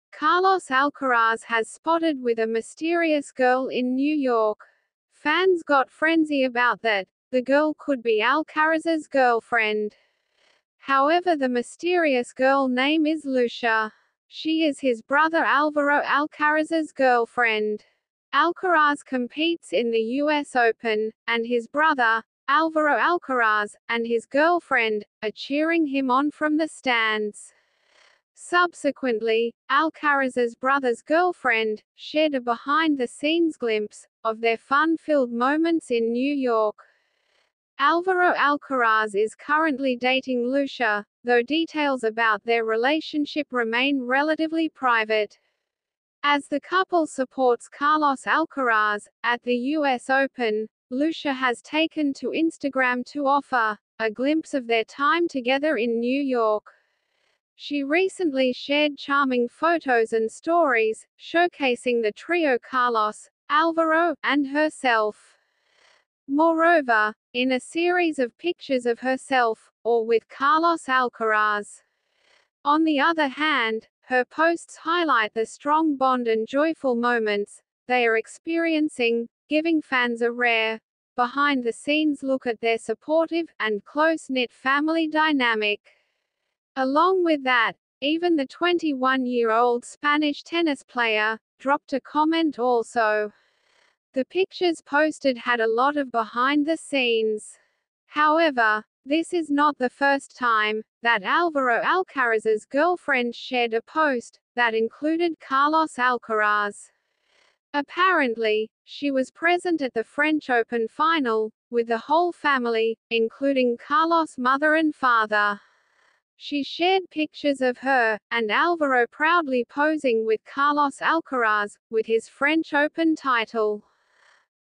Carlos Alcaraz spotted with a "mysterious" girl in New York city
Carlos Alcaraz has spotted with a mysterious girl in New York. Fans got frenzy about that The girl could be Alcaraz's girlfriend. However The mysterious girl name is Lucia. She is his brother Alvaro Alcaraz's girlfriend.